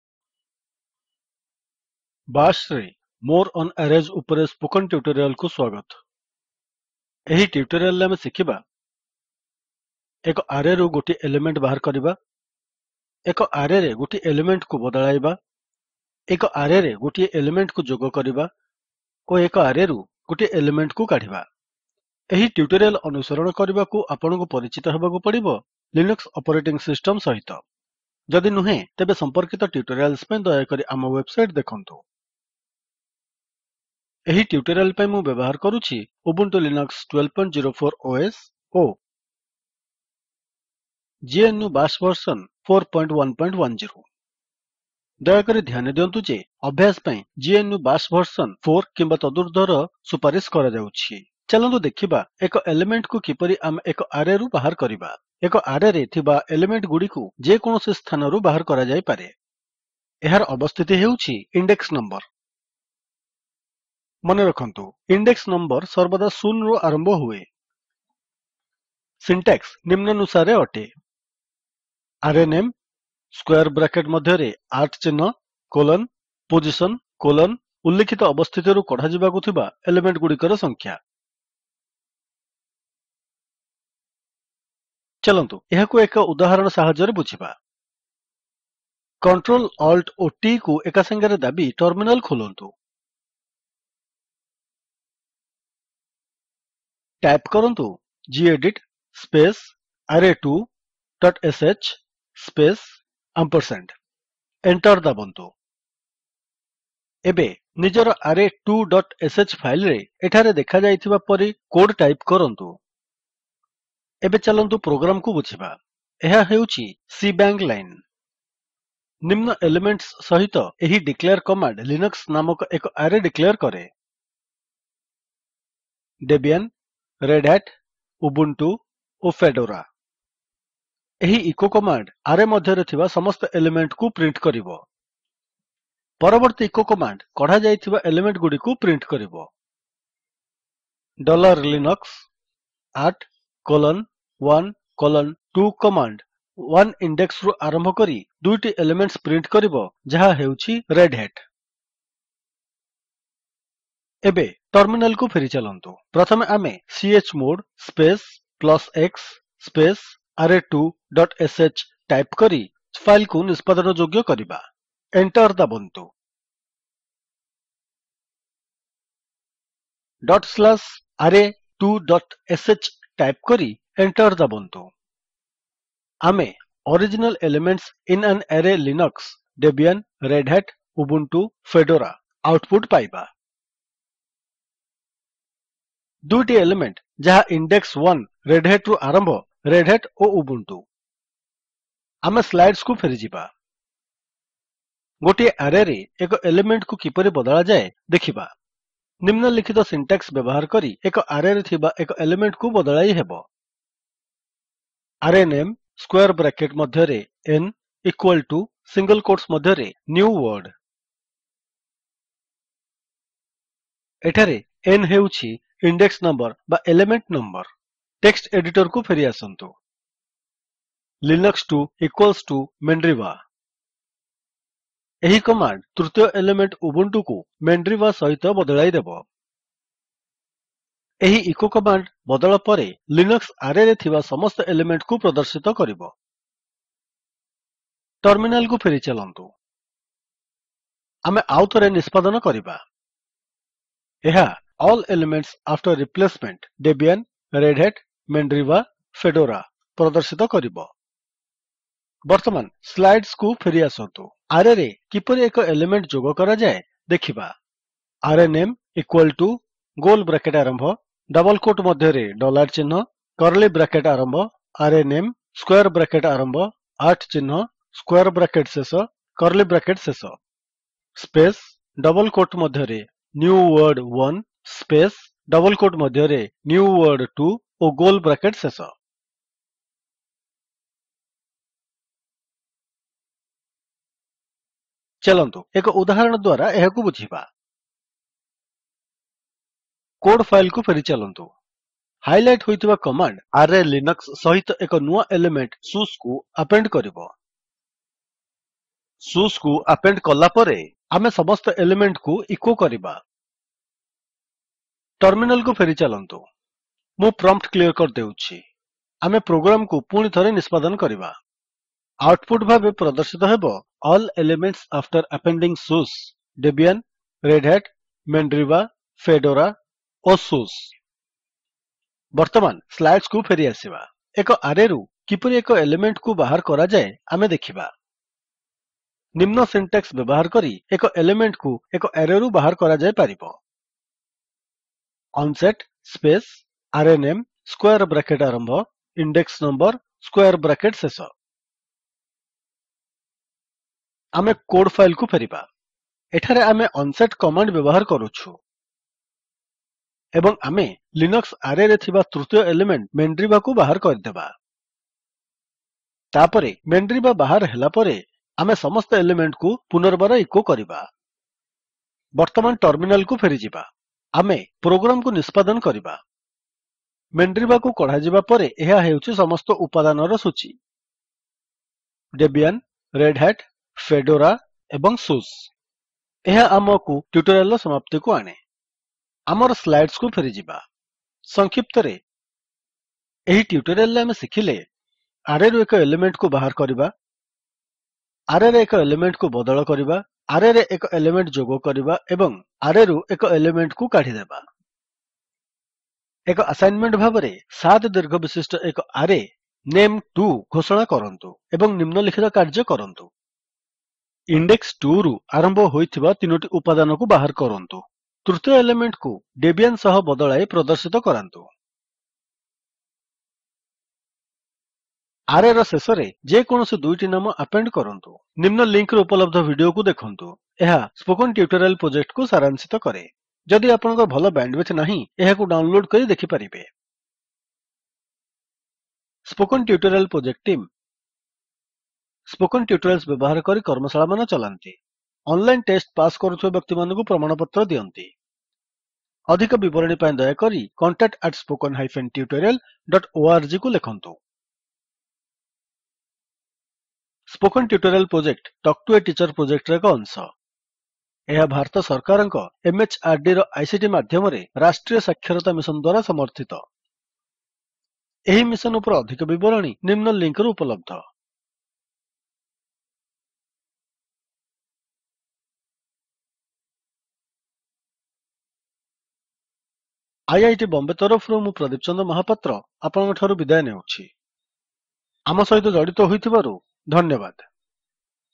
More on Arrays - Oriya
More on Arrays

        ଆରେର ଏଲିମେଣ୍ଟଗୁଡିକୁ ବାହାର କରିବା
        ଗୋଟିଏ ଆରେ ଏଲିମେଣ୍ଟରେ ଖୋଜିବା ଓ ବଦଳ କରିବା
        ଏକ ଆରେରେ ଗୋଟିଏ ଏଲିମେଣ୍ଟ ଯୋଗ କରିବା
        ଏକ ଆରେରୁ ଗୋଟିଏ ଏଲିମେଣ୍ଟକୁ କାଢିଦେବା
        କନସେପ୍ଟକୁ ବୁଝାଇବା ପାଇଁ ଶେଲ୍ ସ୍କ୍ରିପ୍ଟ